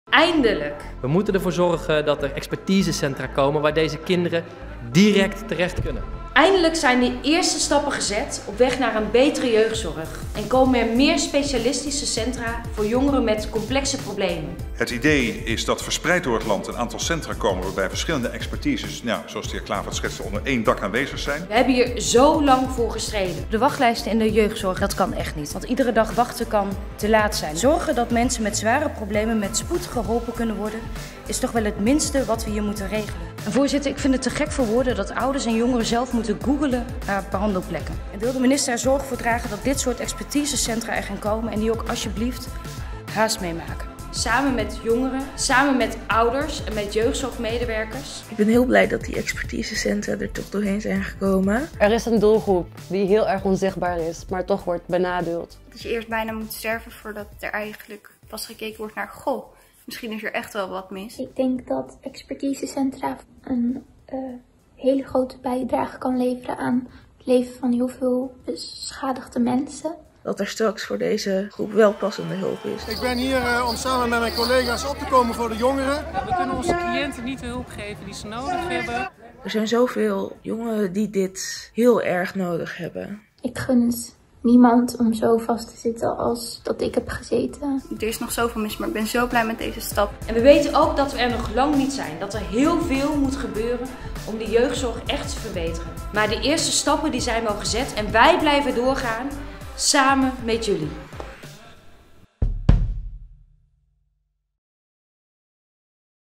De eerste stappen voor een betere jeugdzorg zijn gezet | Lisa Westerveld | 2019
Lisa Westerveld heeft hard gestreden voor een betere jeugdzorg in Nederland en met succes! De eerste stappen zijn gezet om kwetsbare jongeren met complexe problemen te helpen in expertisecentra. Wij blijven doorgaan samen met jullie!